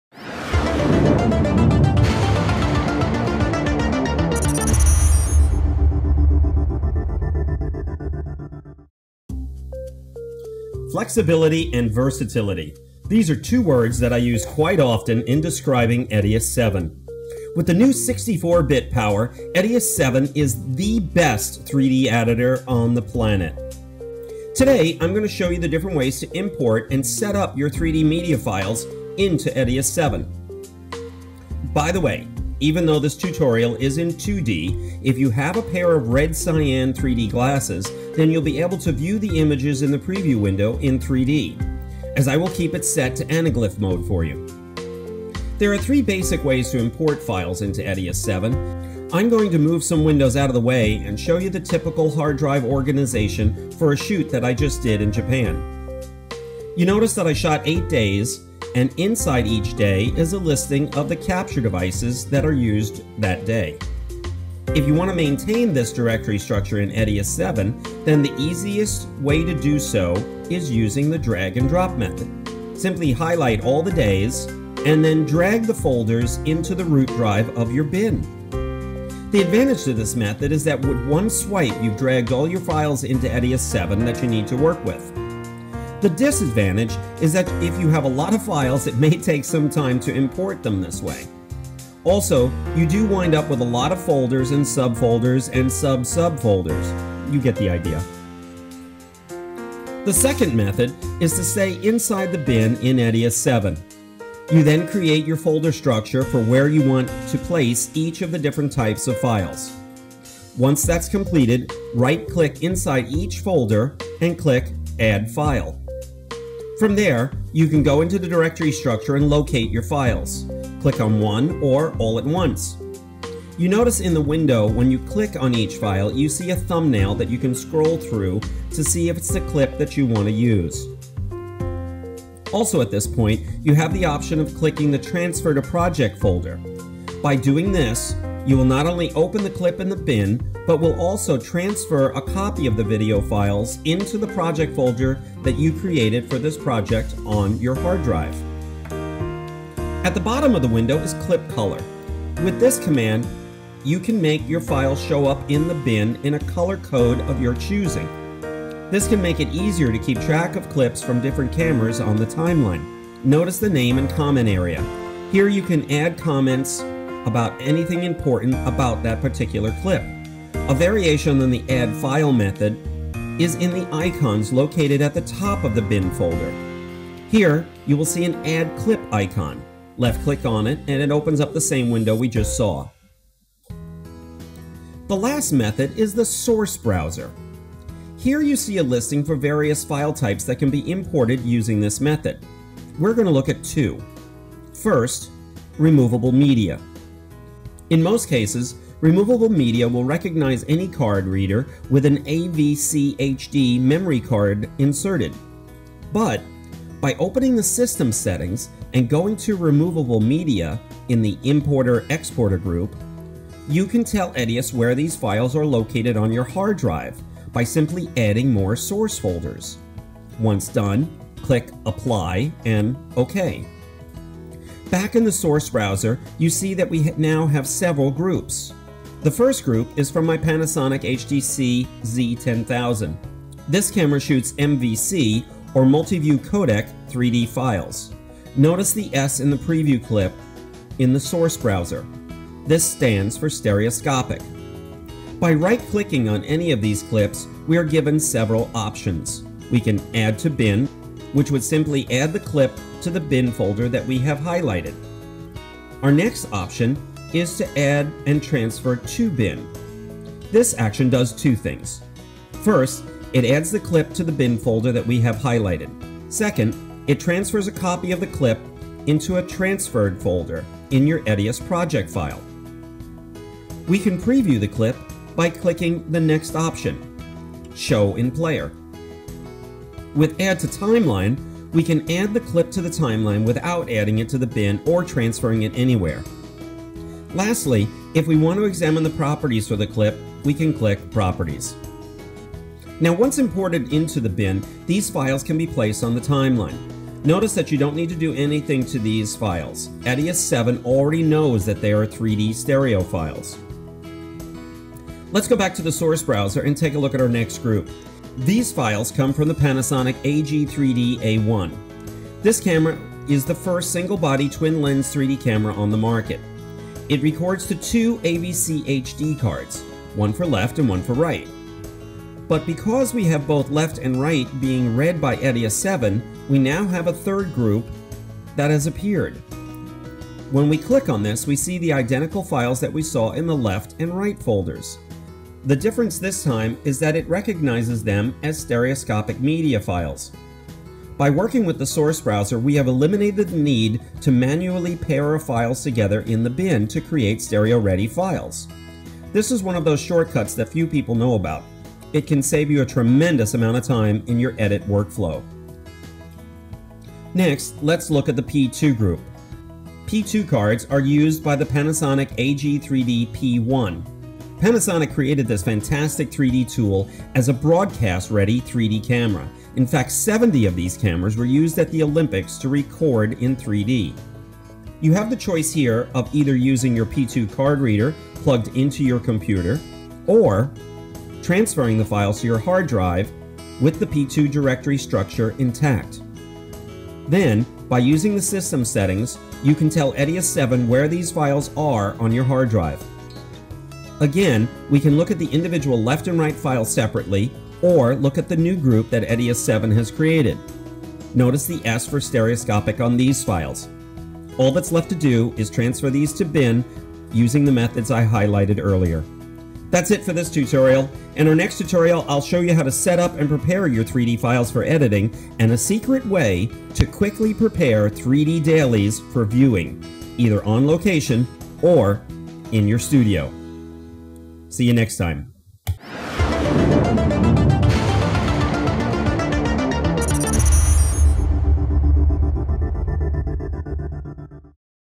Importing 3D Files into EDIUS 7
Learn some very important tips and tricks to enhance your 3D workflow. Using actual 3D media files from a variety of 3D cameras you will understand the importance of proper file structure, multiview codec (mvc) files, and high bit-rate SBS files.